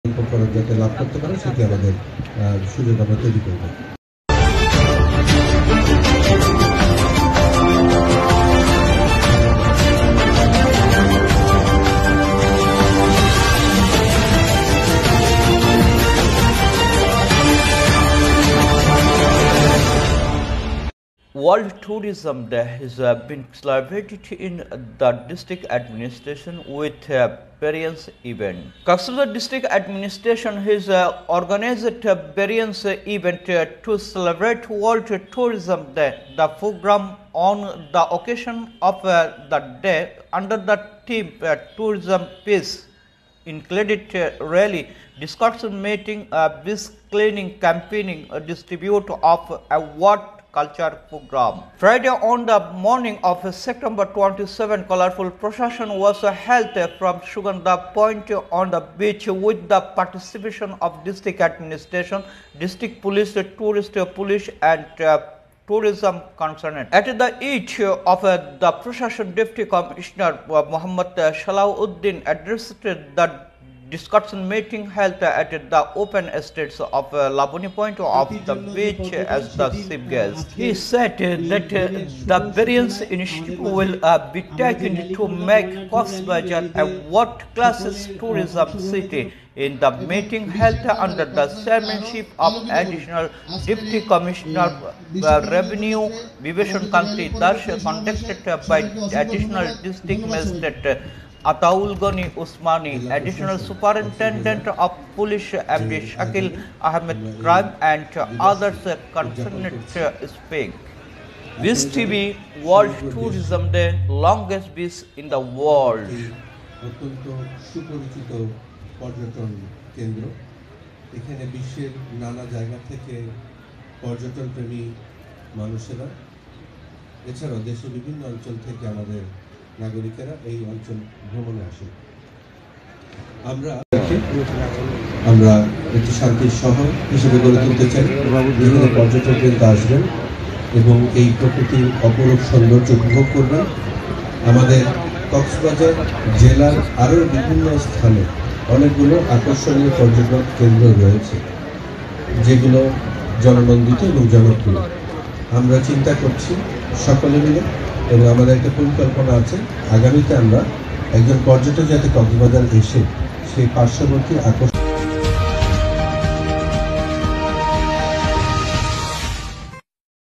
World Tourism Day has been celebrated in the district with various events
Cox's Bazar district administration has organized various events to celebrate World Tourism Day. The program on the occasion of the day under the theme 'Tourism Shanti Sopan' included rally, discussion meeting, beach cleaning campaign, distribution of awards, cultural programs.

(Friday) On the morning of September 27, a colorful procession was held from Sugandha Point on the beach with the participation of District Administration, District Police, Tourist Police and Paraton concerned.

At the end of the procession, Deputy Commissioner Mohammad Salah Uddin addressed the discussion meeting held at the open stage of Labani Point of the beach as the chief guest. He said that various initiatives will be taken to make Cox's Bazar a world-class tourist city. In the meeting held under the chairmanship of Additional Deputy Commissioner Vibhishan Kanti Das, Additional Superintendent of Police Md Shakeel Ahmed and other concerned spoke.
1:07 PM
Mri Shohel
Mri Shohel Khan